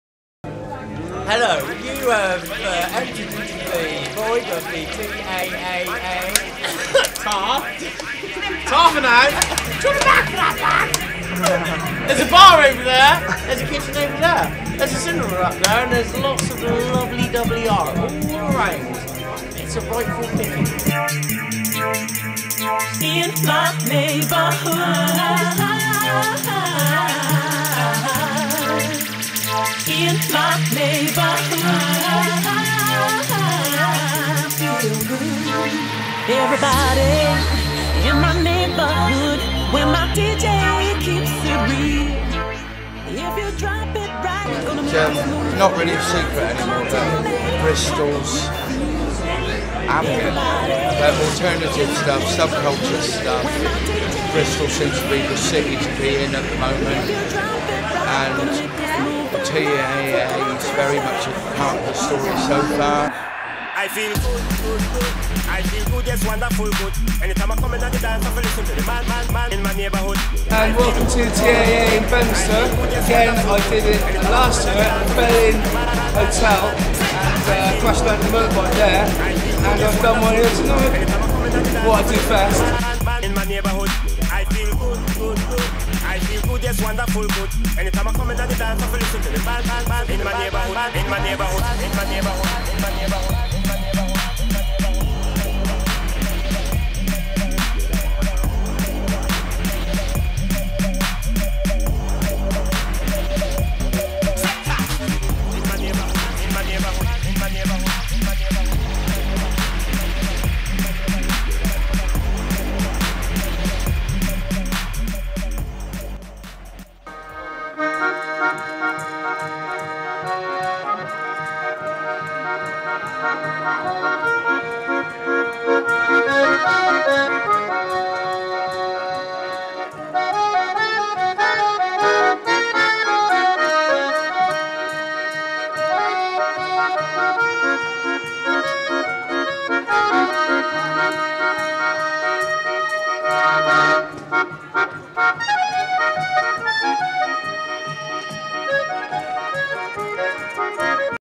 TAA - Bedminster, Bristol (first rough edit for documentary pitch)
First rough edit taken from the Temporary Autonomous Art exhibit at the old bingo hall in Bedminster. 31st march & 1st april. have alot more to edit, but this was a quick vid i put together for my documentary presentation I done for uni on my genre research.
sadly never got to shoot on the final day as i was told it was closed down on the saturday, and also had my boom mic nabbed and lost my iphone on the friday to find out people was uploading pics onto my facebook so i was pretty f*@king pissed off to be fair and wasnt up for it!